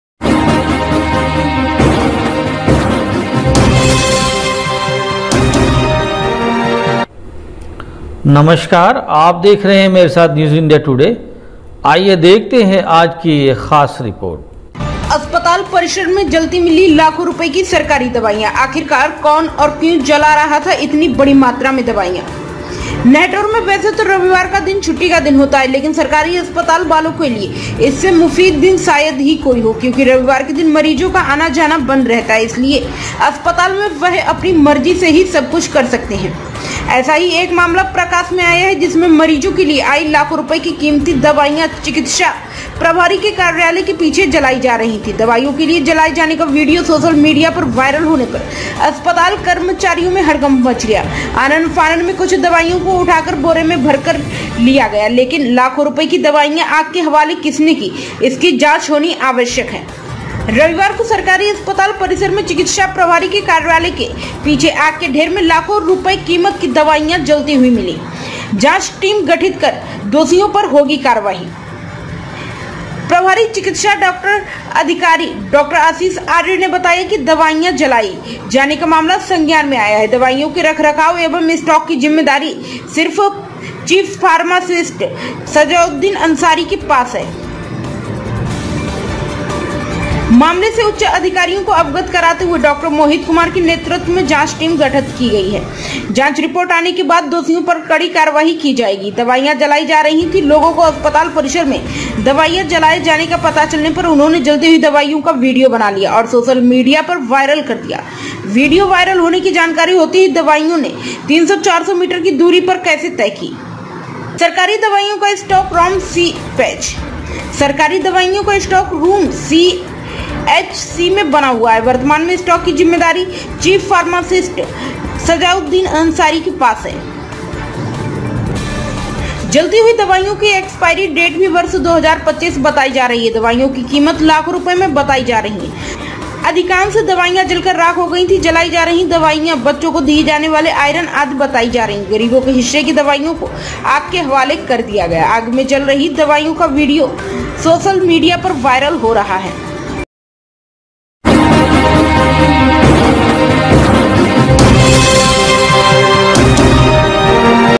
अस्पताल परिसर में जलाई गई लाखो रुपये की दवाइयां,प्रभारी ने की जांच की बात
#@newsindiatoday breking#